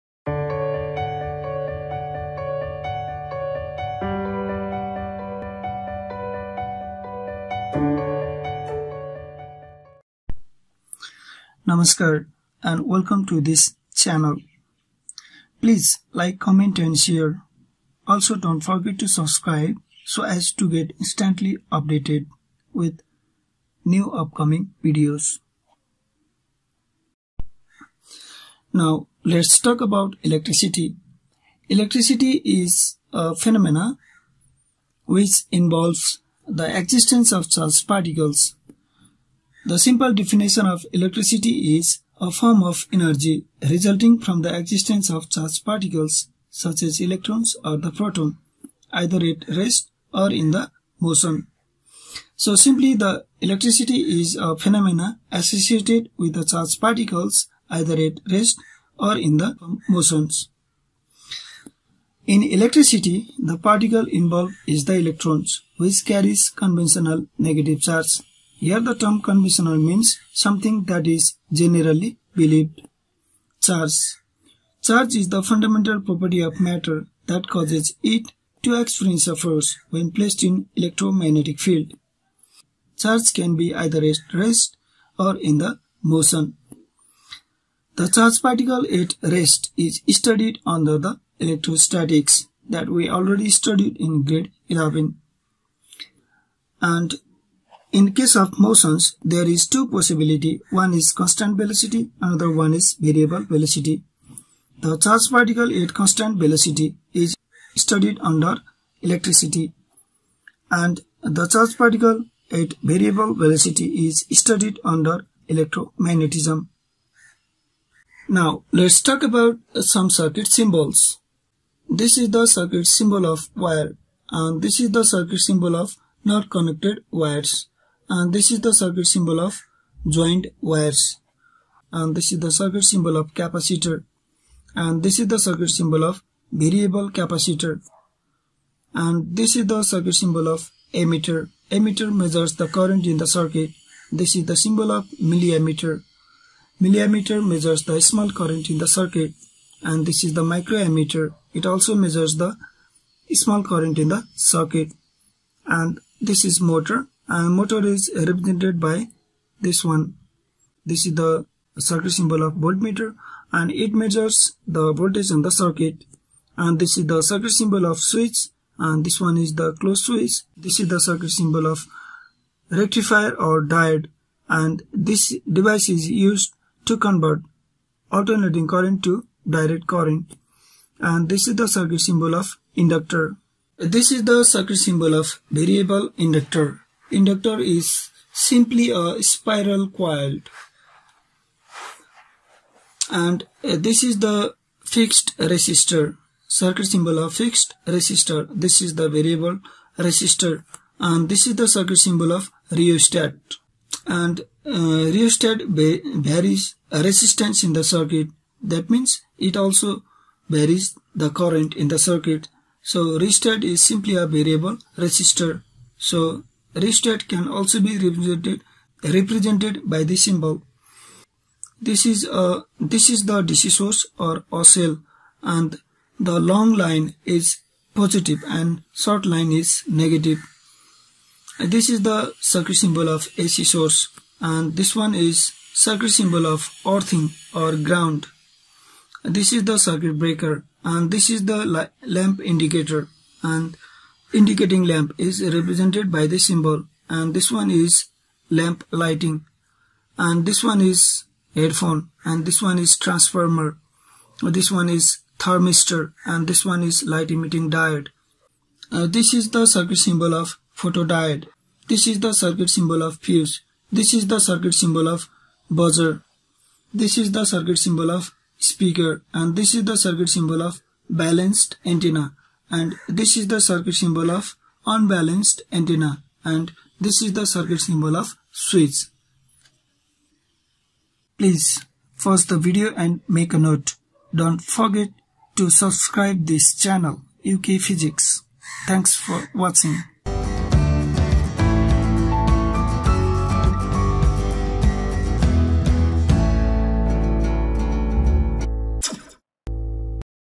Electricity - DC Circuits - Part - 1 - Circuit Symbols : Note
This Tutorial video includes some Electric Circuit Symbols required to understan the basics of electricity.